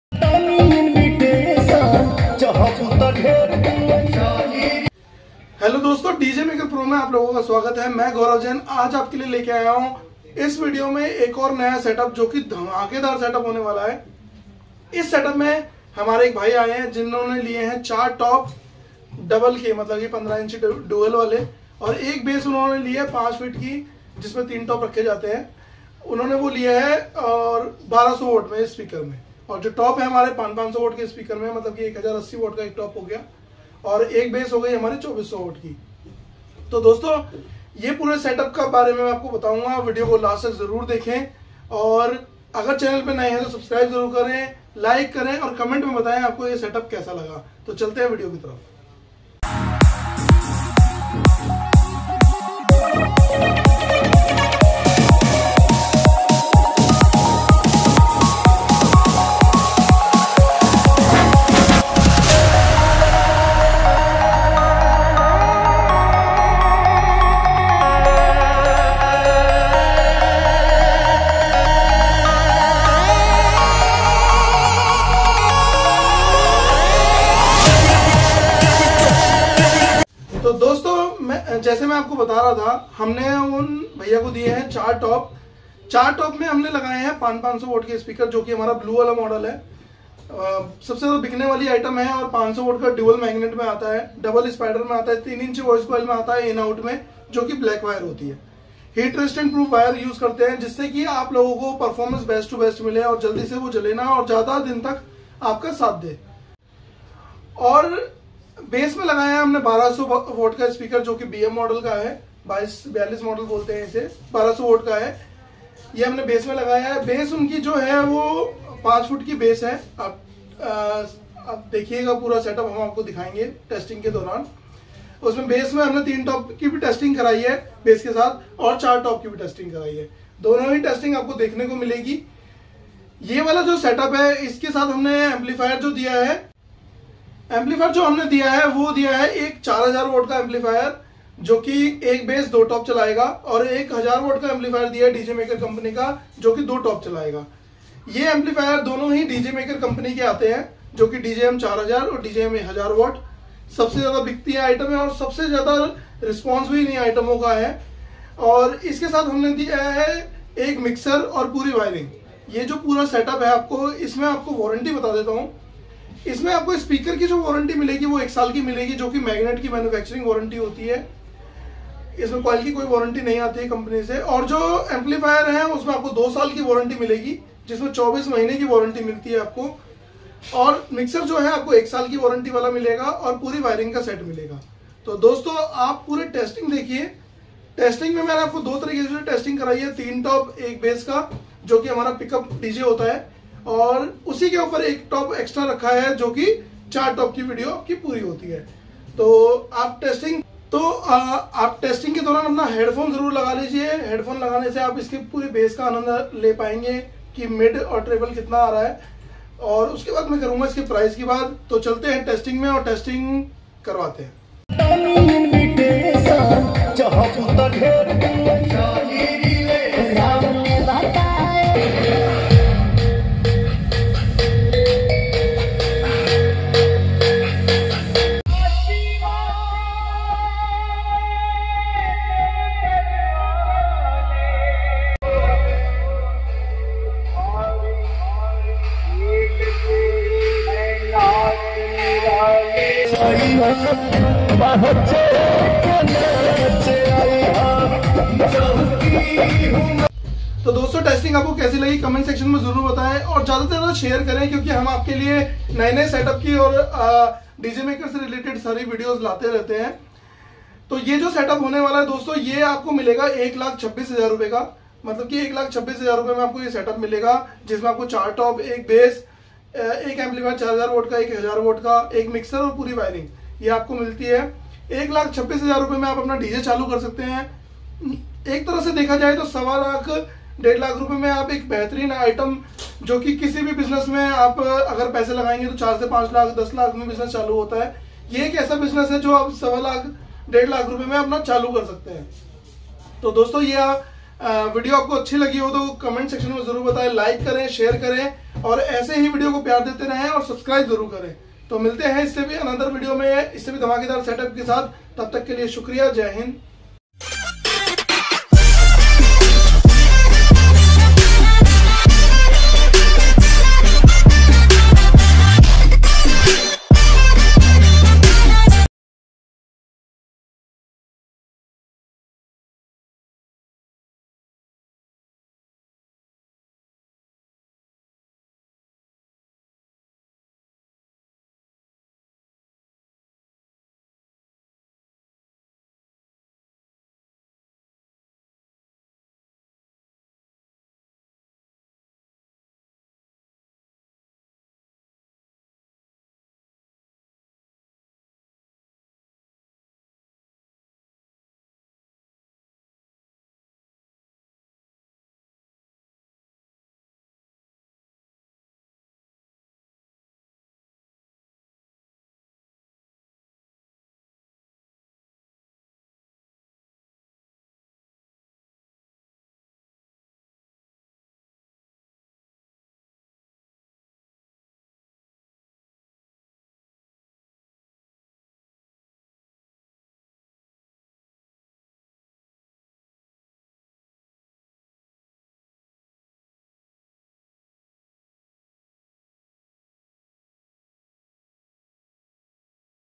PICKUP डीजे SET UP 1100W TOP 2400W BASS 🔥 मात्र ₹ 126000 DJ सेटअप टाइगर dj
DJMAKER AUDIO JBL
djmakerpro

4top
1bass
2 amplifier
1 mixer

MRP ₹126000

Instagram par follow करे
ADHIKLAL_YADAV

Shop address :-
DJMAKER AUDIO
572, old Lajpatrai market, opposite RED FORT
Delhi- 110006